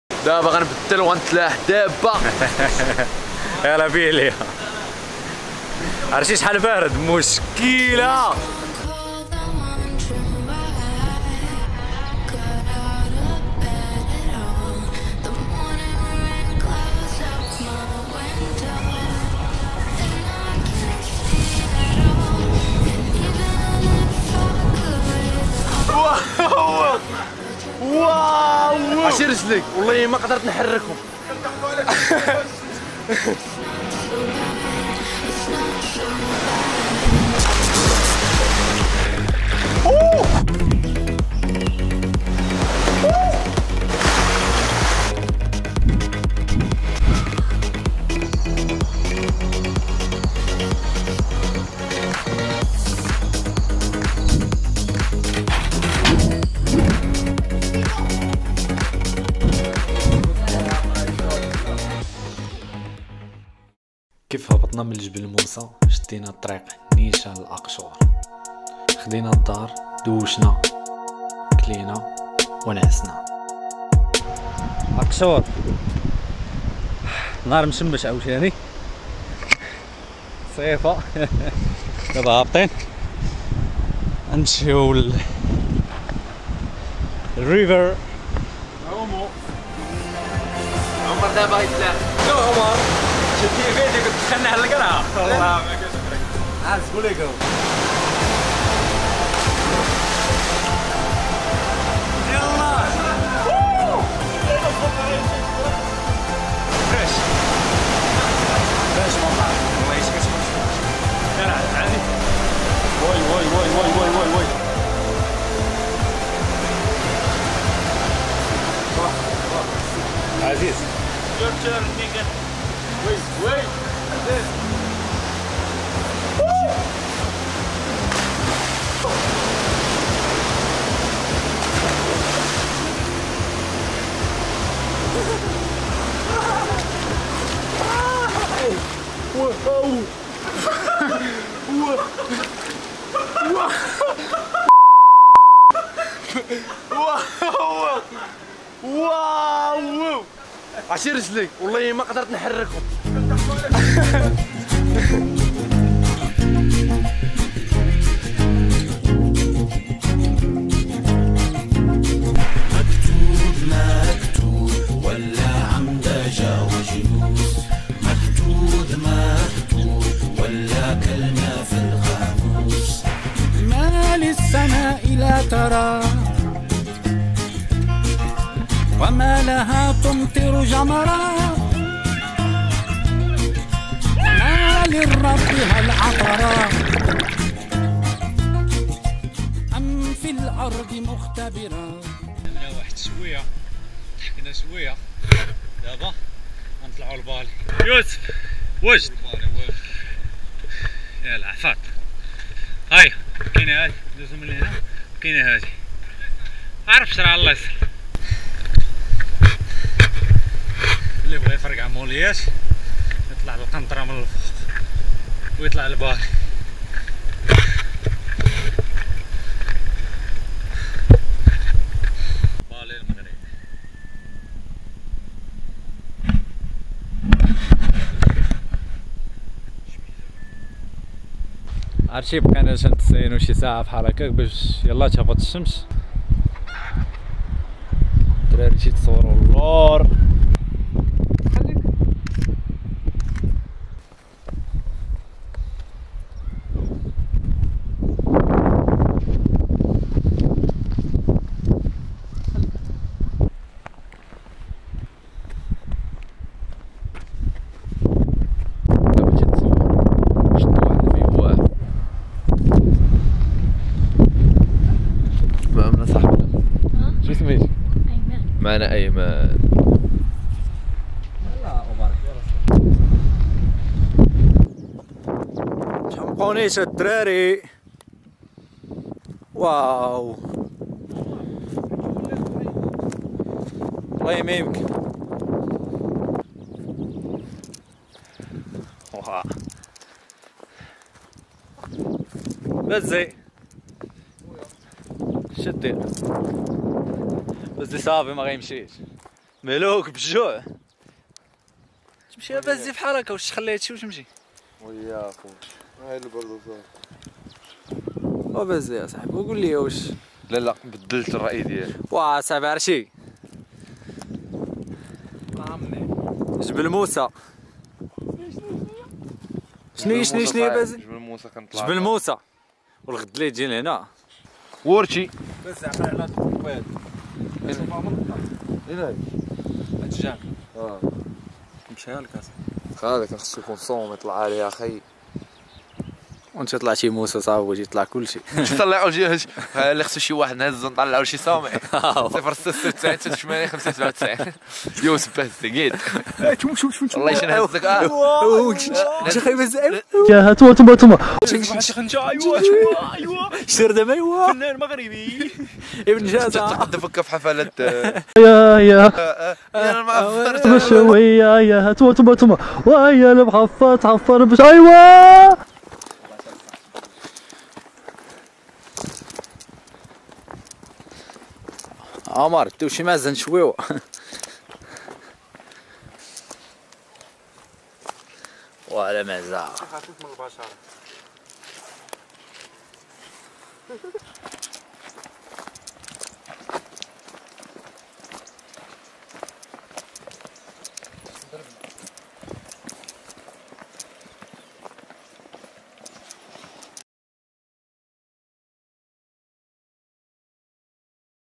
Akchour Chefchaouen (vlog ) | أنا متاكد معمرك شفتي هاد بلاصة من قبل ف أقشور
After an amazing night at the summet of Jbel Moussa we took our to Akchour to enjoy our journey swiming at one the natural pools.  we discovered the Moroccan Bali that people didn't know about and its just in Akchour, contact me on my INSTAGRAM ACCOUNT for more informations.